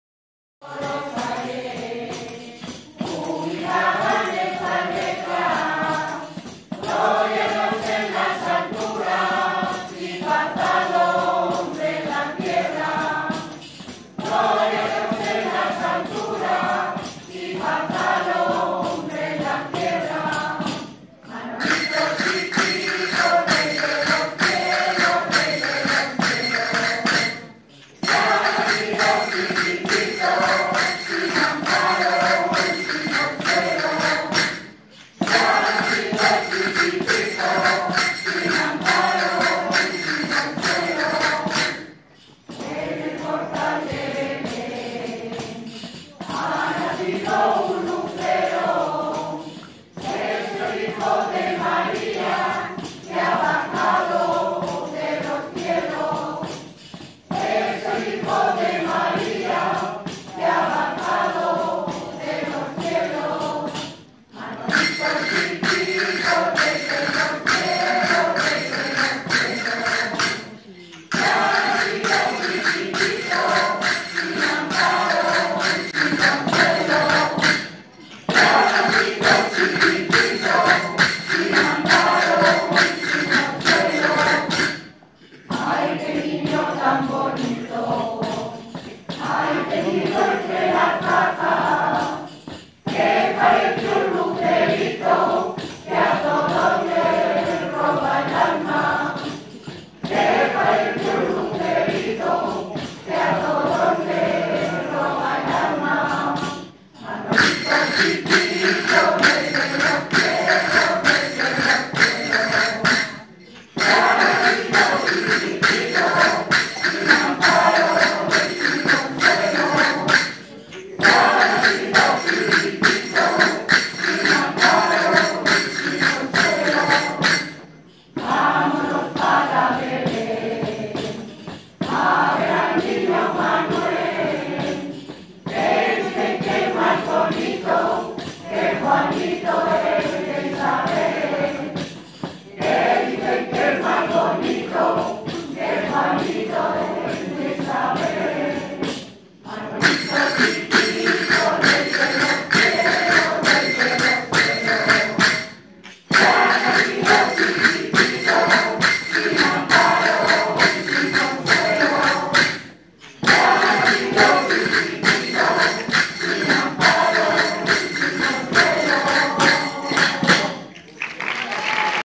Gala Navideña El Angel 6.
Gala Benéfica de Navidad de la Asociación El Ángel de Zamora.